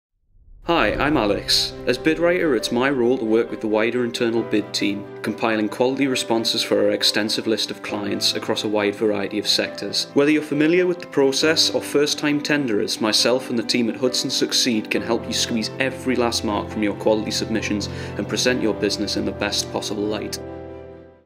Alex Holt | Hudson Employ
Hudson Welcomes A New Bid Writer
Today we welcome, Bid Writer, Alex Holt to the Hudson Succeed team. Alex will work to support the delivery of our bid writing services.

On a daily basis, Alex will work alongside our team of Bid Managers, Bid Writers and our Head of Bid Management. Together, they will continue to support both national and international businesses, varying in size and industry. As a team of consultants, Alex will adapt his writing constantly to ensure that the industries are represented as accurately as possible.

Alex will also liaise with our clients to ensure high-quality communications throughout the tendering process.

Alex Says…
“As a Bid Writer at Hudson Succeed, I will be working to develop quality submissions for our large client base. My role will involve securing new contracts for our ever-growing clientele. I look forward to making a positive contribution over the next year and beyond and the whole team have already made me feel very welcome.”

Previous Experience
Alex graduated from Newcastle University with a BA Hons Degree in English Language. During his time at university, Alex developed an interest in persuasive language and neuro-linguistic programming. This soon became the focus of Alex’s dissertation.

Whilst undergoing his degree, Alex had regularly produced and updated an online blog surrounding the motor vehicle industry. Alex has a passion for unique cars and his blog allowed him to share his interests with others, driving a 1987 BMW 5-series currently.

Alex has been providing bid writing services for the past two years. Working with a dynamic range of sectors including Grounds Maintenance, Mechanical and Asbestos, Alex has invaluable experience of the procurement industry.

During this time, Alex was required to support a range of businesses and create persuasive tender responses. Alex would also liaise with the internal bid team to ensure that submissions were completed to the highest possible quality.

Our Head of Bid Management Says…
“We are delighted to welcome Alex to the Hudson Succeed Team. Alex is a talented and experienced Bid Writer. His industry knowledge and previous experience will be incredibly valuable to the team as we are constantly expanding the sectors that we operate in” – Daniel Hall.